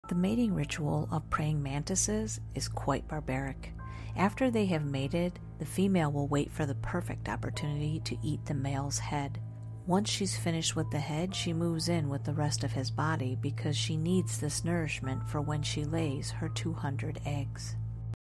Scary Facts About Praying Mantises You Didn't Know 😨
Here are some scary facts about praying mantises you didn't know about...

If you would like to send me a scary story to be possibly whispered in the future, you can send your story to the email listed below.  If we pick your story, we will contact you and advise you.